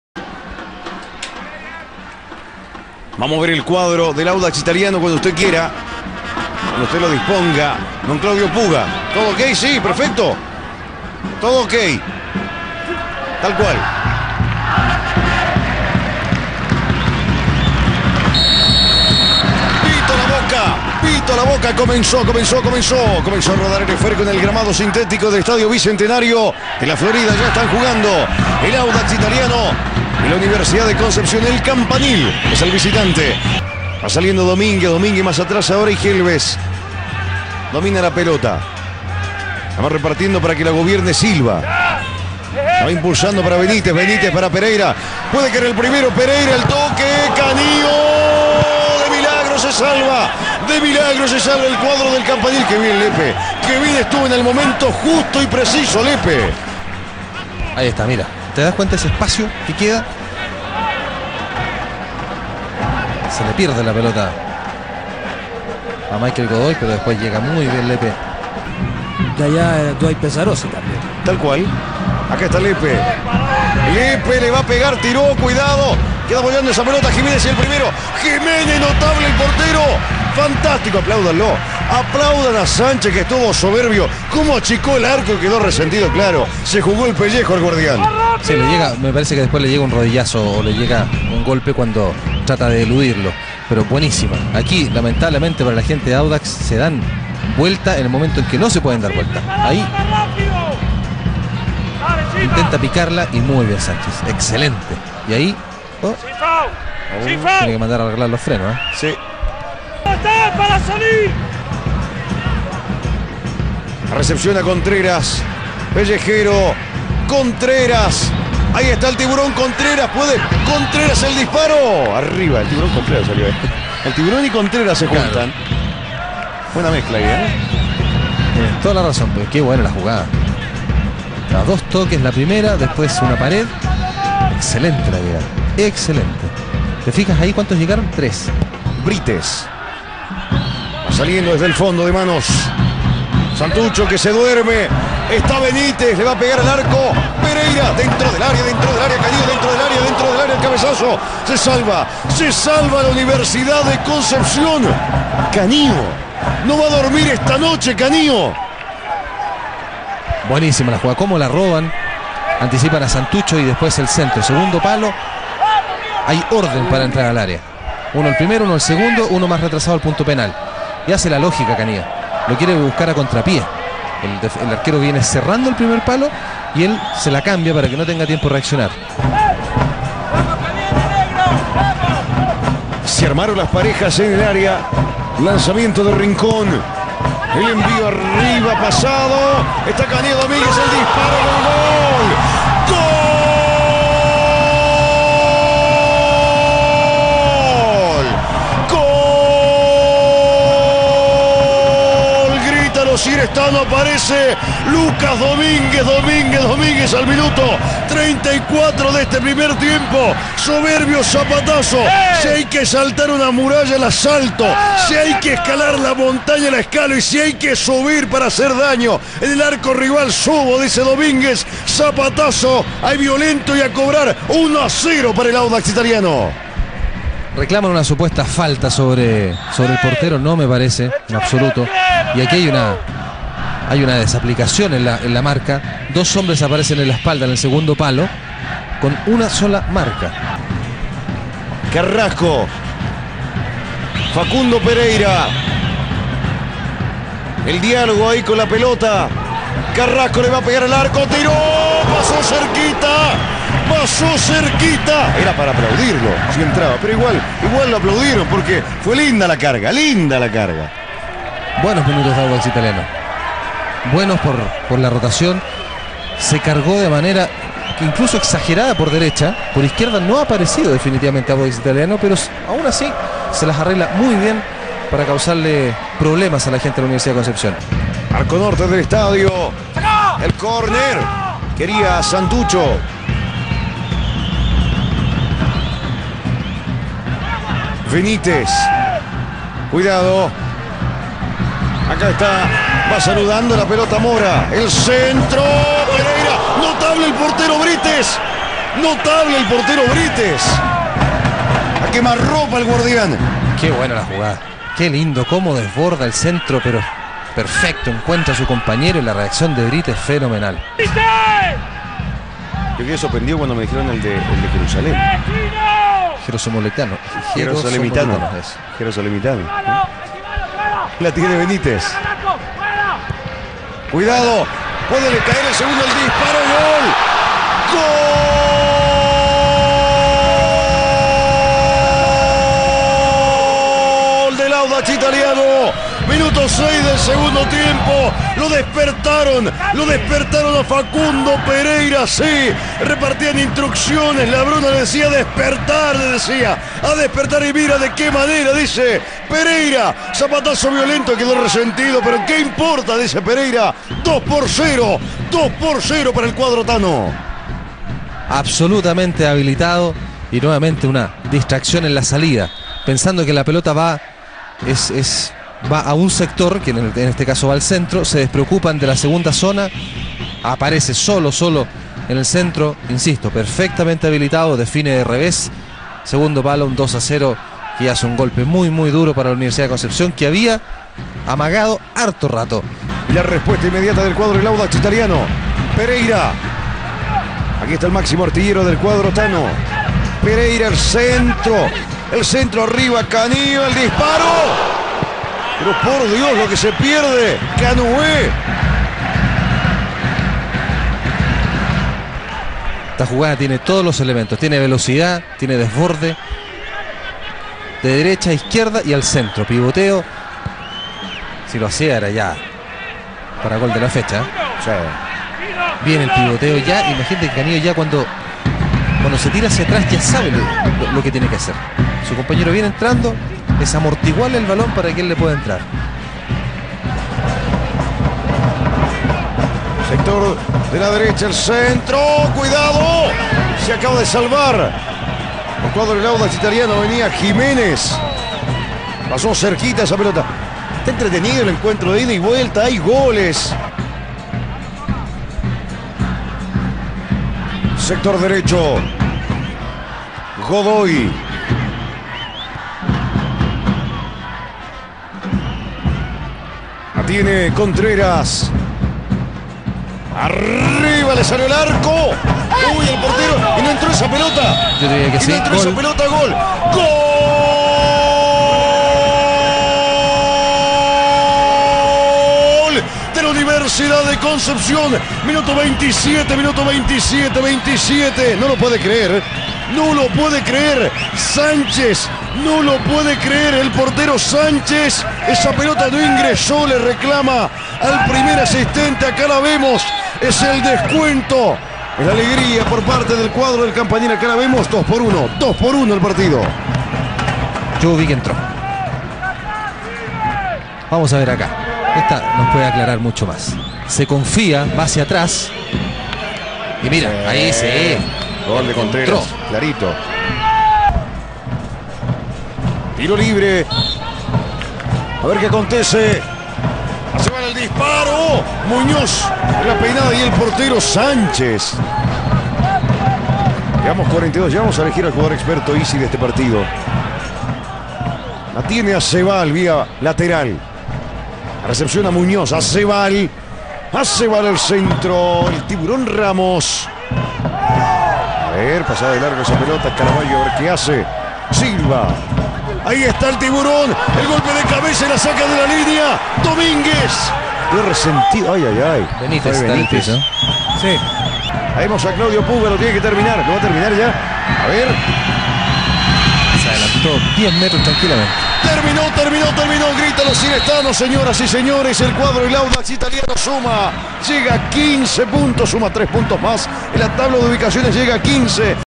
A. Italiano 2-1 U. de Concepción (Clausura 2011 - Fecha 8)
1-0 Lucas Domínguez
2-0 Facundo Pereyra
2-1 José Contreras